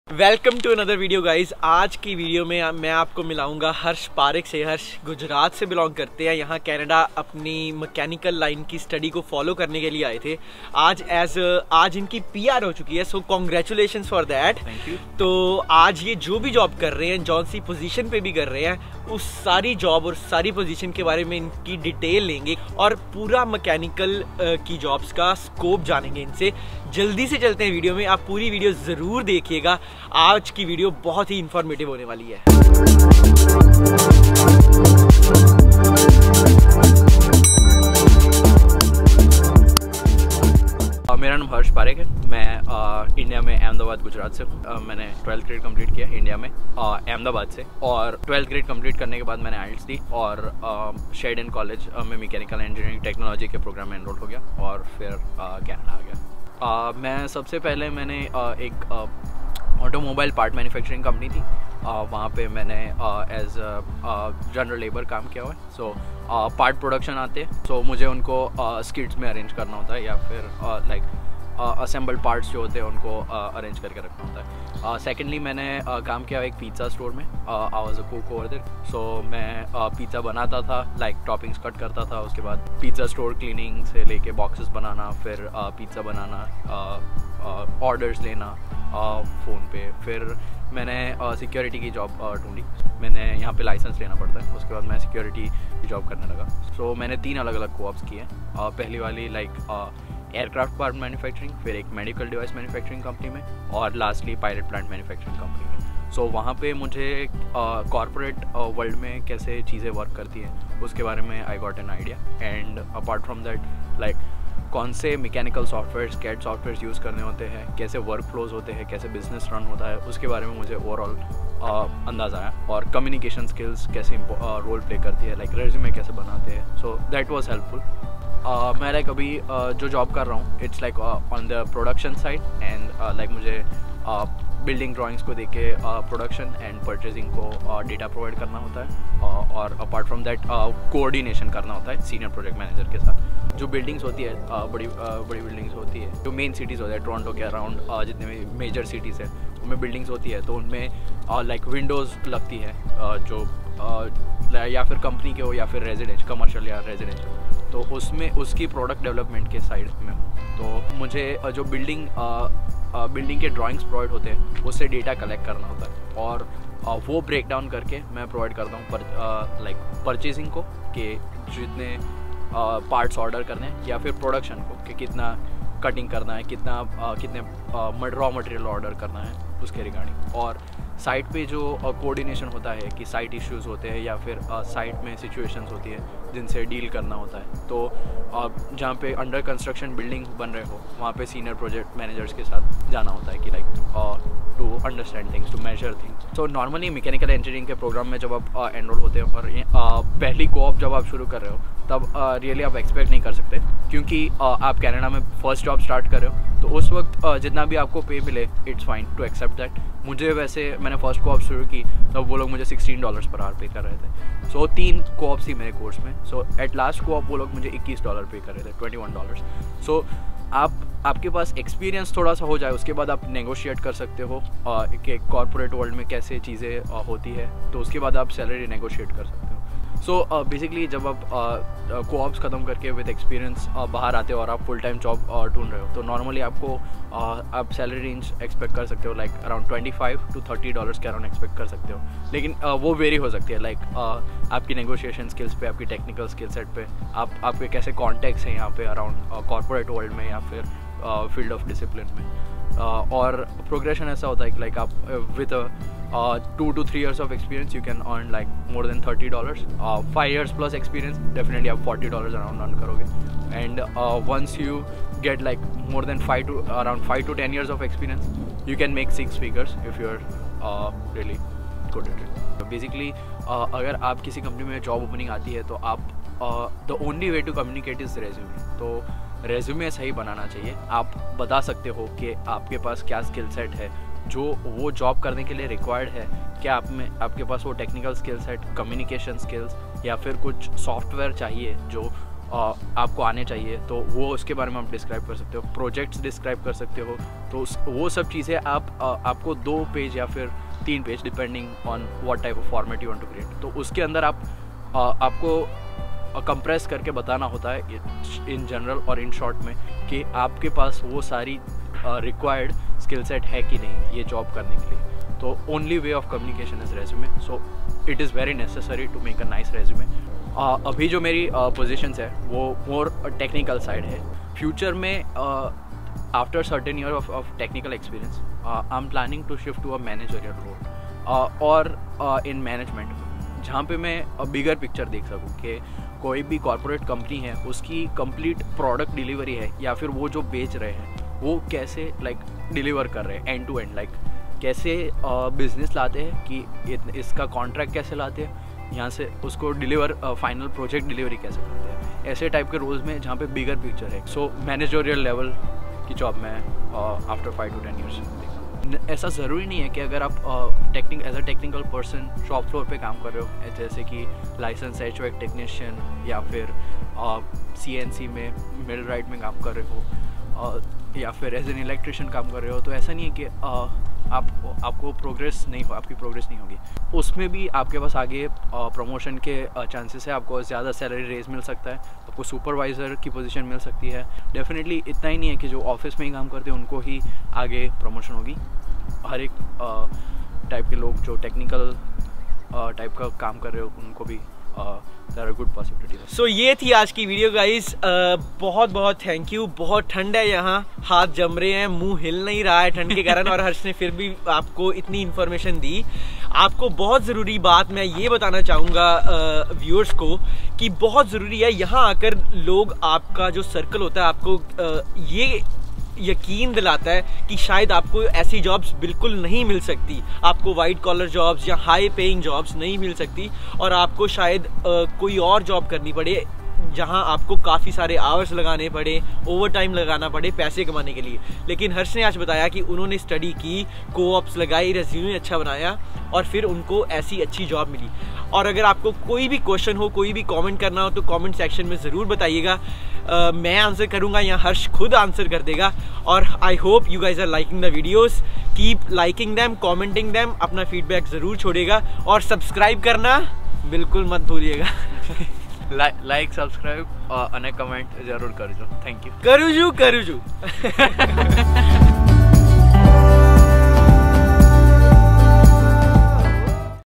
Mechanical Engineering Jobs in Canada | Scope of Engineering | International Student | New Immigrant
Hi,

In this video, you will get to know the experience of Harsh Parekh, who came to Canada as an International Student and is working as a Process Engineer in the Mechanical Engineering related field today.

Hope you all will enjoy it.

Best,
Abhishek

Chapters:
0:00 - Introduction
0:41 - Background
0:57 - Job History in Canada
1:37 - Co-op Experience
2:08 - Job Description
3:16 - Salary Expectation
4:33 - Importance of Resume
5:20 - Future Scope as an Engineer
5:59 - Future Scope as a Technical Person
6:36 - Important tip for new students and immigrants